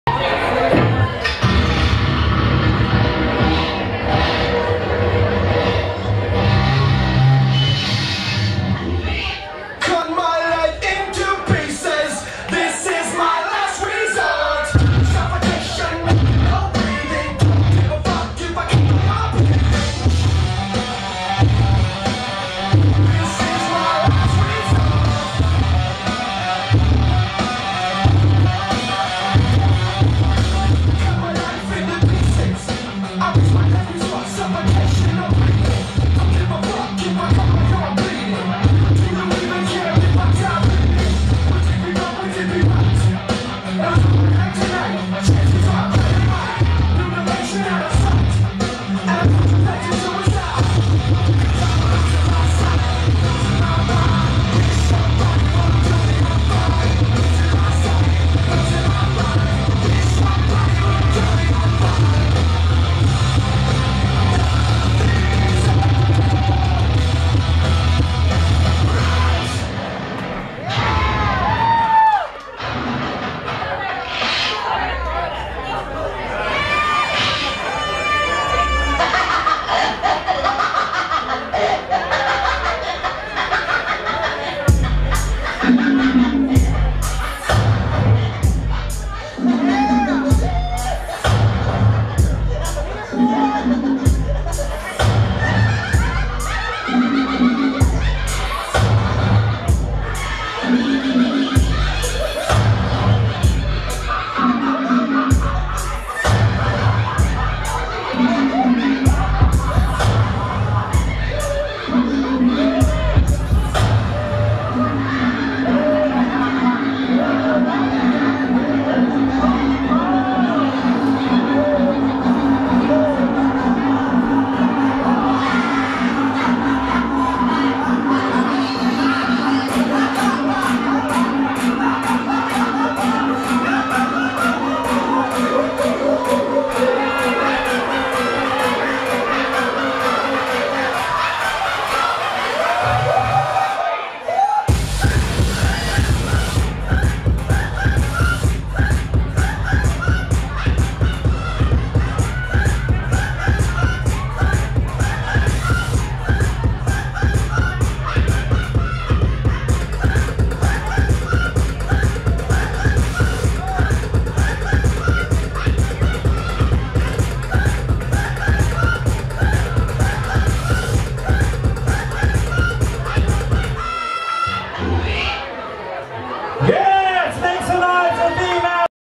V Master Chad (Hallowest III Extra Gory Edition)
Hallowest III District of the Damned extra gory night! District West in Columbus, OH